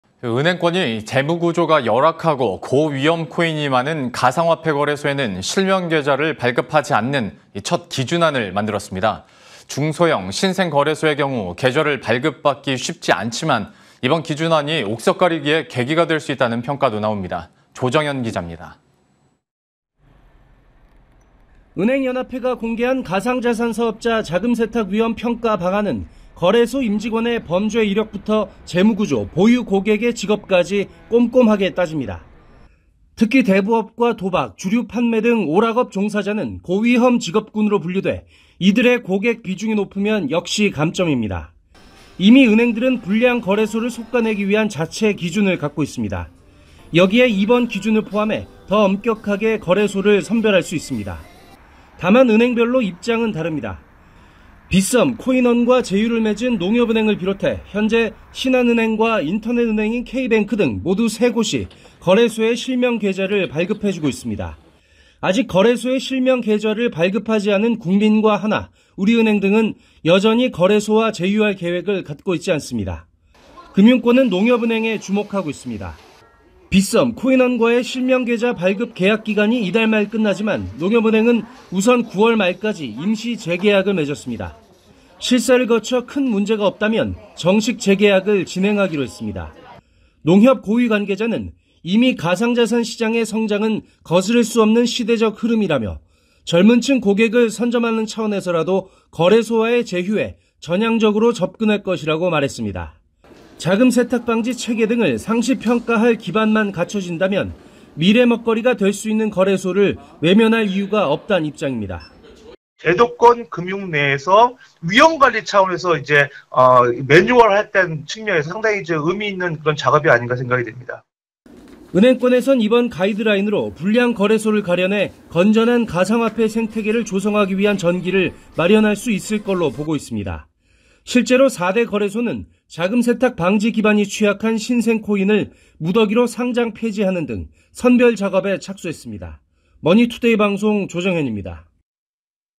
도박·주류판매는 감점…은행권 가상화폐 거래소 옥석가리기 시동 / 머니투데이방송 (뉴스)
07/09 이슈&뷰11

은행권이 재무구조가 열악하고 고위험 코인이 많은 가상화폐 거래소에는 실명계좌를 발급하지 않는 첫 기준안을 만들었습니다. 중소형, 신생 거래소의 경우 계좌를 발급받기 쉽지 않지만 이번 기준안이 옥석 가리기의 계기가 될 수 있다는 평가도 나옵니다.
조정현 기자입니다.